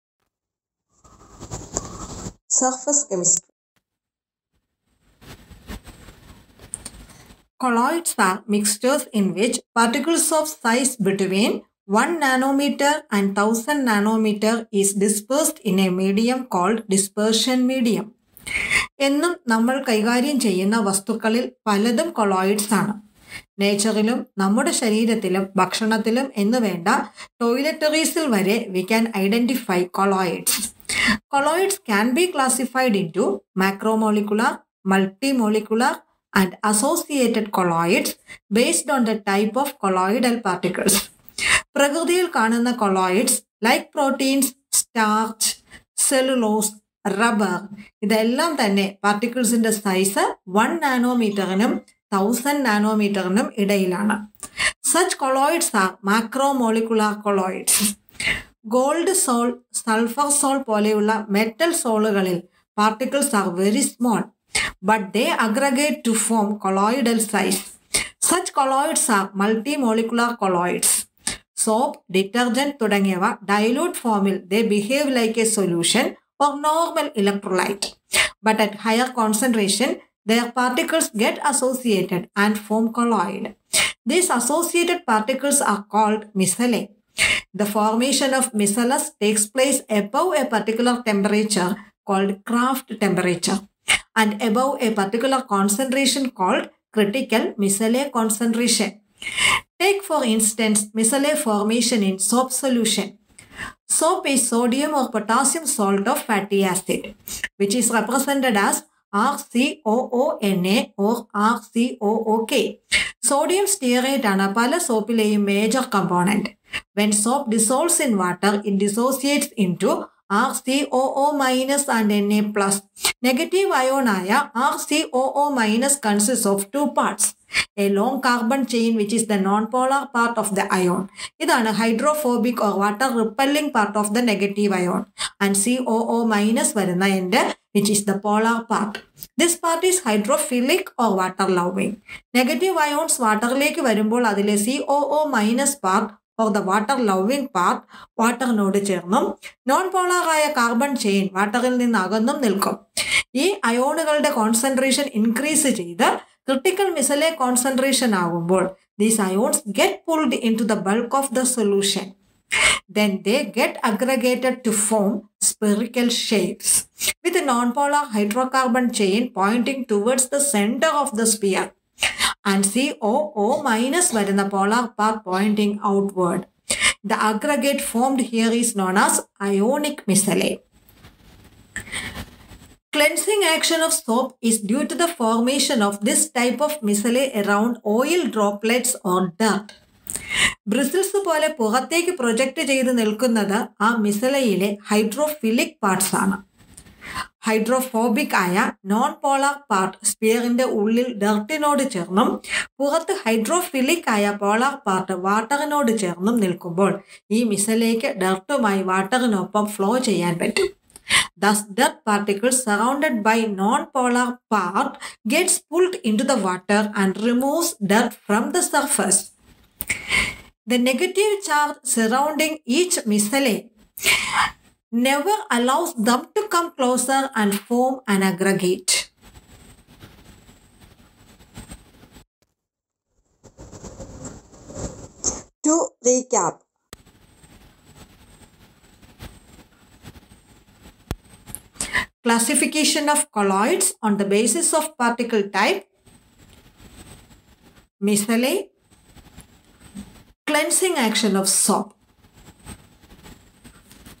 MICELLE FORMATION chapter 5 : surface chemistry
Colloids are heterogeneous mixtures in which the size of particles comes in between 1nm and 1000 nm. Substances like milk, butter, cheese, Proteins, starch etc are colloids. Soaps and detergents are associated with colloids. Their cleansing action is through micelle formation.

Classification of colloids based on the type of particles

Macromolecular colloids 00:32

Multimolecular colloids 00:32

Associated colloids 00:32

Micelle 01:26

The cleansing action of soap 1:43